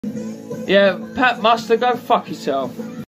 Freddie beans calls out Pat Mustard
This man is an alcoholic and druggie that wakes his kids up on a regular basis